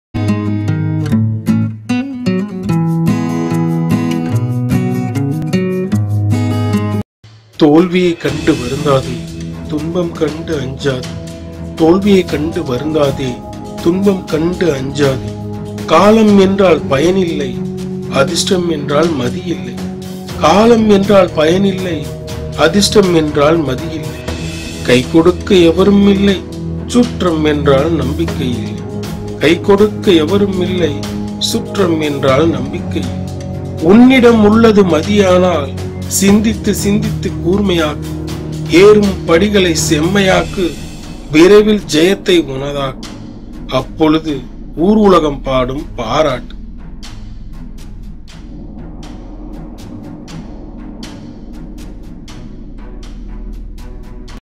நம்பிக்கை - தமிழ் கவிதை Nambikkai
தோல்வியை கண்டு வருந்தாதே!
துன்பம் கண்டு அஞ்சாதே!

காலம் என்றால் பயனில்லை,
அதிர்ஷ்டம் என்றால் மதியில்லை,